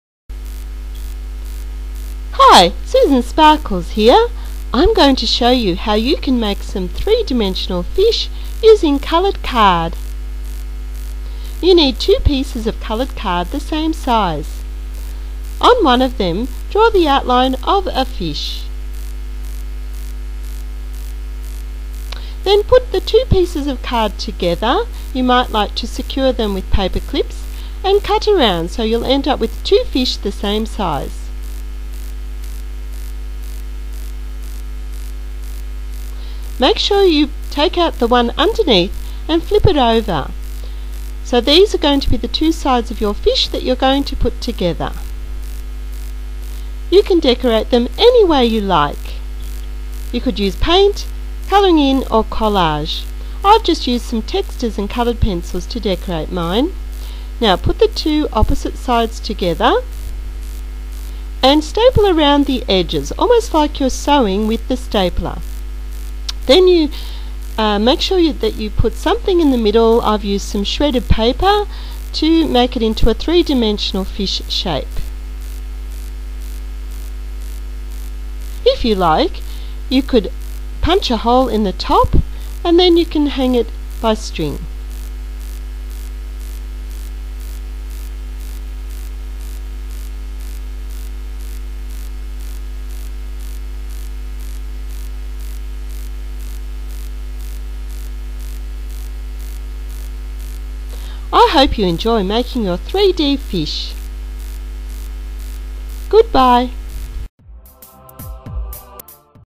Art/Craft Activity - Susan Sparkles makes 3D fish
Susan Sparkles shows how you can make a three dimensional fish from coloured card.